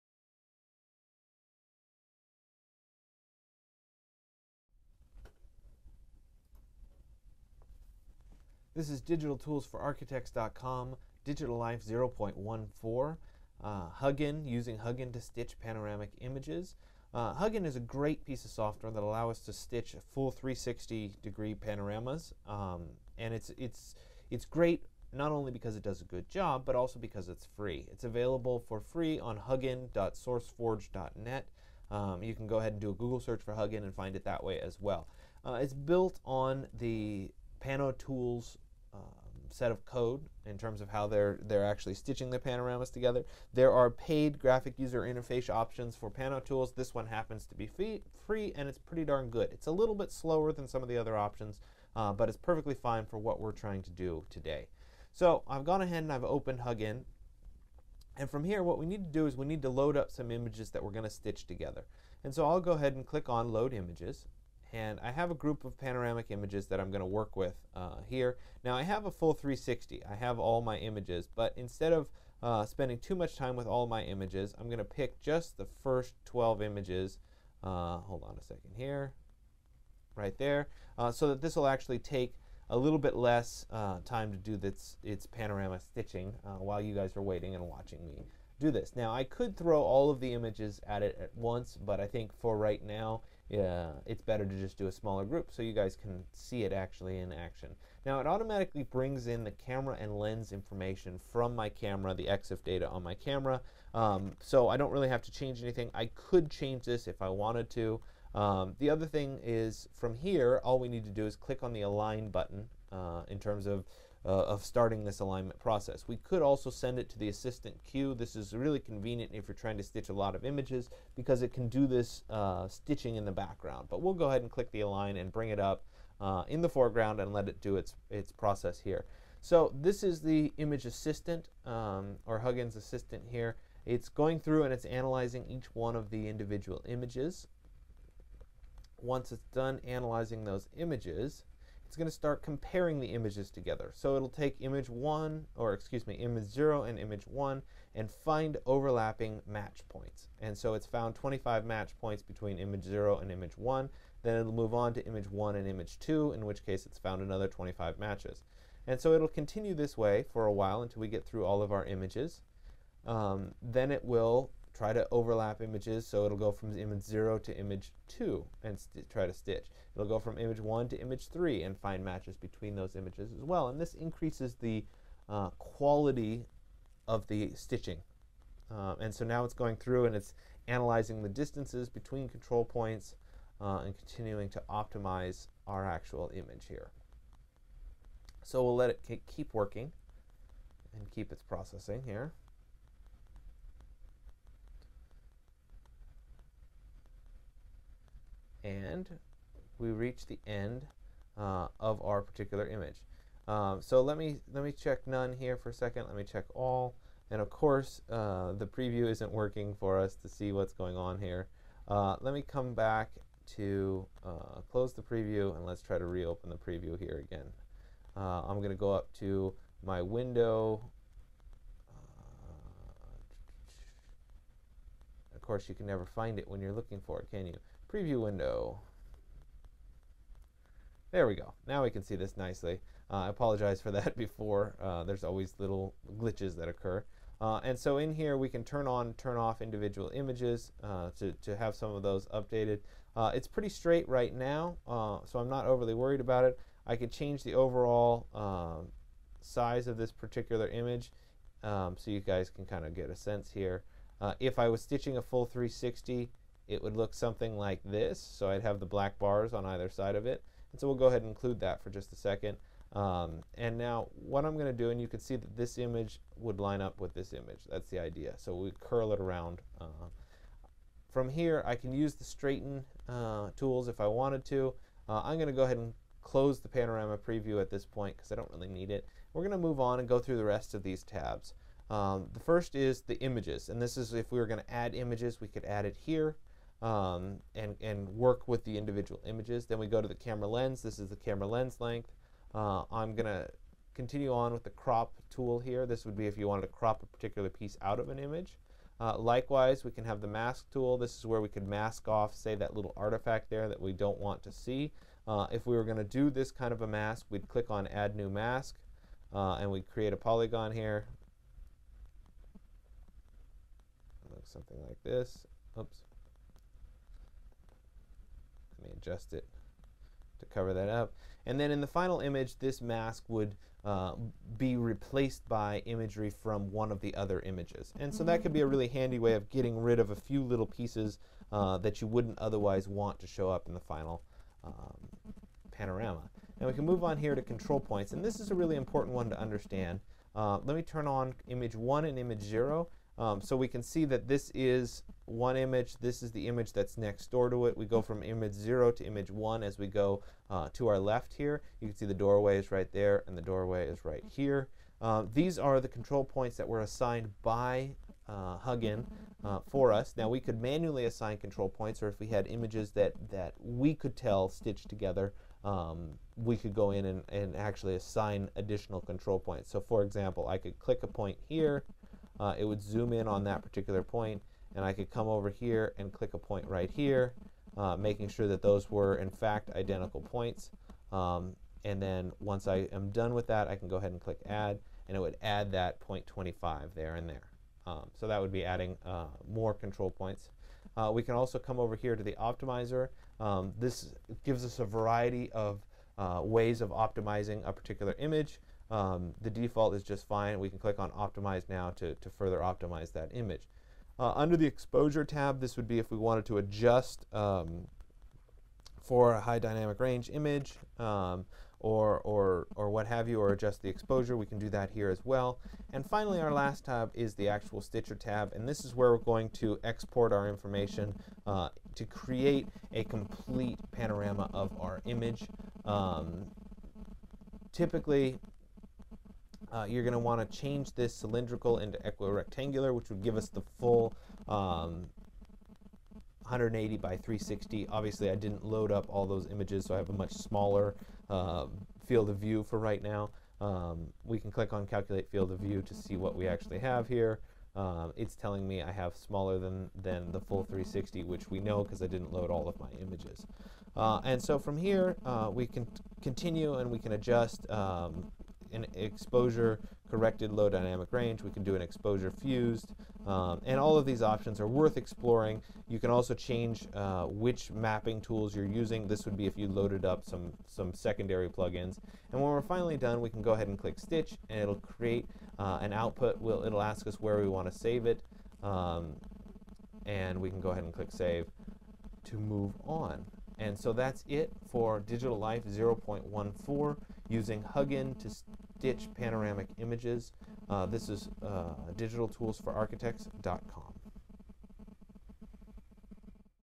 DTFA Digital Life 0.14 [hugin]
Using Hugin to stitch a panoramic (360 degree) image.